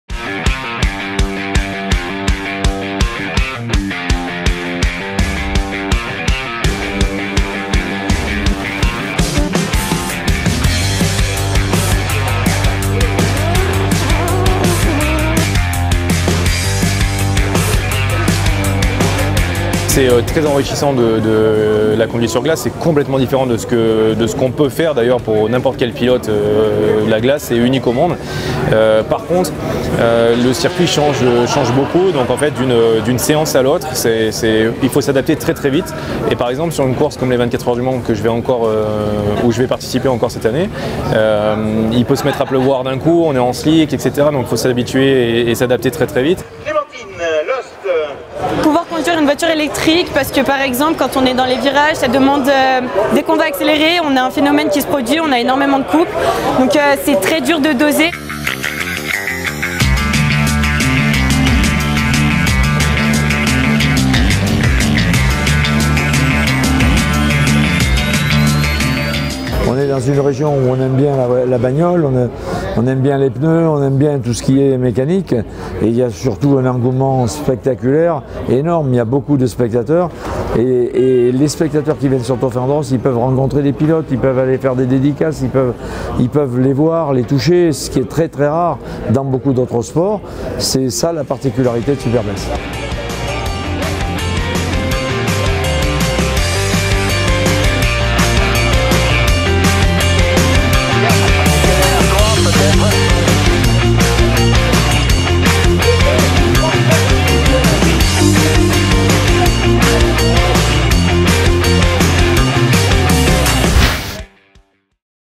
Trophée Andros-Clermont/Superbesse 2019 :
C'est sur six étapes que le 30ème Trophée Andros réunit cette année, 30 pilotes dans trois catégories : Enedis Electrique, Elite thermique et Elite Pro où des voitures électriques et thermiques de 340 chevaux à quatre roues motrices, s'affrontent ensemble pour la première fois. A Super-Besse, le 2 février 2019, témoignages de deux pilotes , Clémentine Lhoste et Nathanaël Berthon, un clermontois soutenu par la Ville de ClermontFerrand. La municipalité clermontoise apporte, en outre, une solide contribution technique pour l'organisation de l'événement.